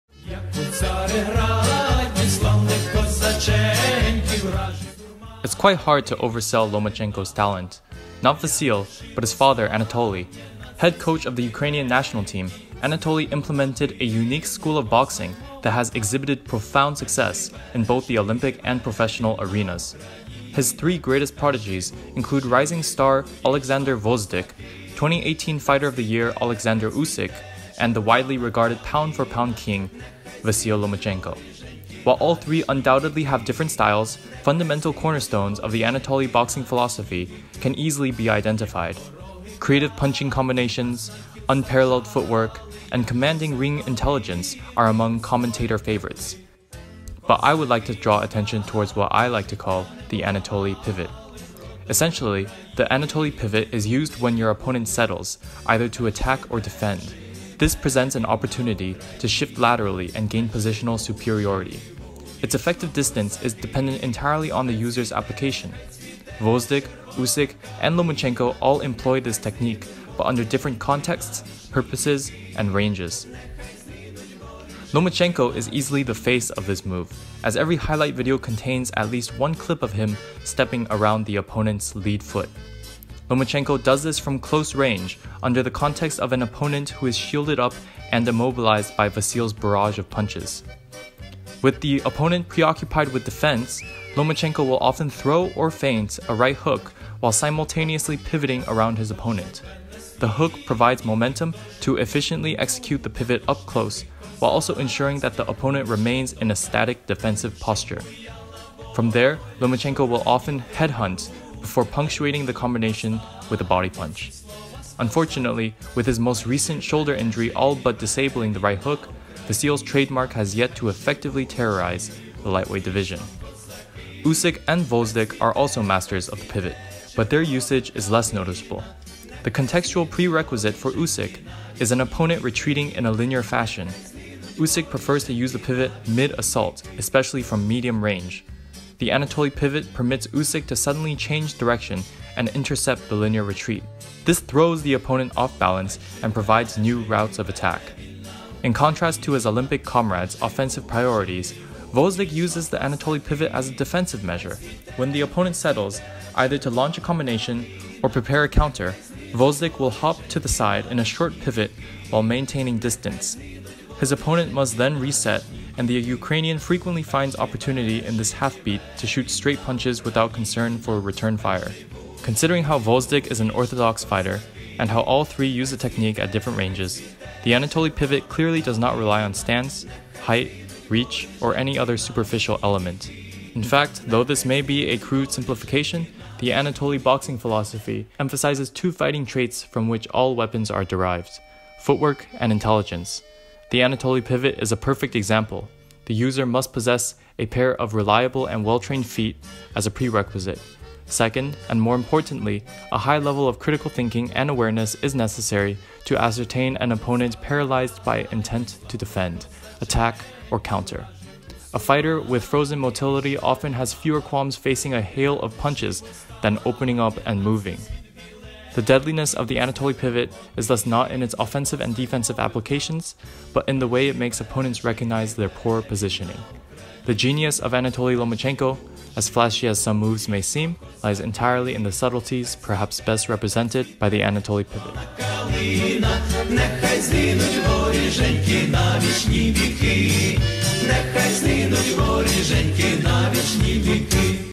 Anatoly Lomachenko: The Anatoly Pivot
A brief analysis on one of boxing's greatest trainers: Anatoly Lomachenko. In this IGLUprofile, I specifically examine the Anatoly pivot and how Gvozdyk, Usyk, and Lomachenko exploit its advantages despite each possessing a vastly different style.

Note: While Usyk's application of the pivot does not rely on a static opponent, a fighter who has been forced to retreat straight backwards is not much better than one who is standing still. Settling is not defined by a lack of motion, but rather by ineffective defensive tendencies to guard blindly or back up linearly. Committing to either is equivalent to "settling."